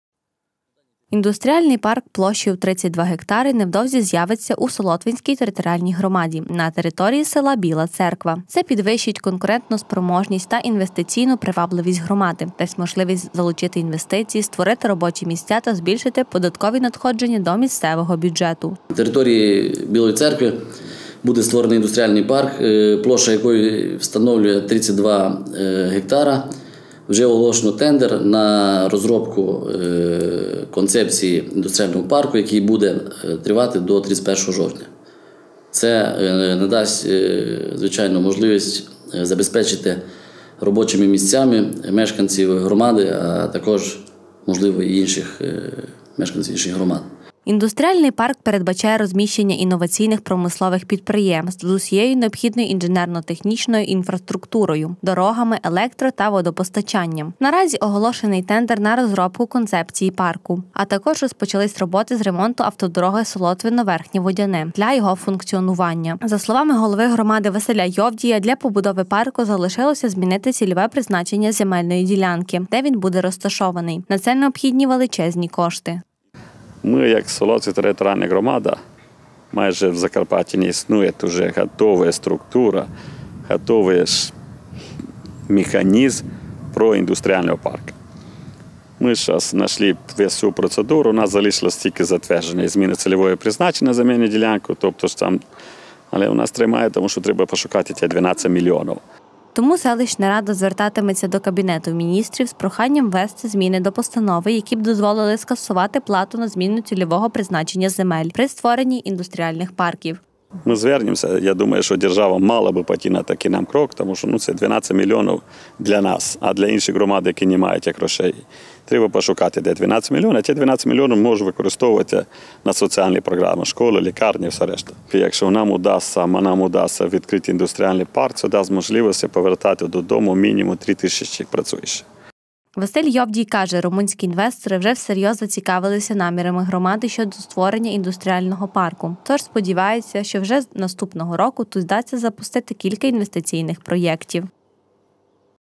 Будують індустріальний парк. Інвестиції та робочі місця
Інвестиції в економіку, робочі місця та розвиток промисловості – у Солотвинській територіальній громаді планують створити індустріальний парк. Вже оголосили тендер на розробку його концепції. Індустріальний парк площею у 32 гектари невдовзі з’явиться у Солотвинській територіальній громаді, на території села Біла Церква. Це підвищить конкурентоспроможність та інвестиційну привабливість громади, дасть можливість залучити інвестиції, створити робочі місця та збільшити податкові надходження до місцевого бюджету.Індустріальний парк передбачає розміщення інноваційних промислових підприємств  з усією необхідною інженерно-технічною інфраструктурою – дорогами, електро- та водопостачанням. Наразі оголошено тендер на розробку концепції парку. А також розпочались роботи з ремонту автодороги Солотвино-Верхнє Водяне для його функціонування.За словами голови громади Василя Йовдія, для побудови парку залишилося змінити цільове призначення земельної ділянки, де він буде розташований. На це необхідні величезні кошти.Тому селищна рада звертатиметься до Кабінету Міністрів з проханням внести зміни до Постанови, які б дозволили скасувати плату за зміну цільового призначення земель при створенні індустріальних парків. Василь Йовдій каже: румунські інвестори вже всерйоз зацікавились намірами громади щодо створення індустріального парку, тож сподівається, що вже наступного року тут вдасться запустити.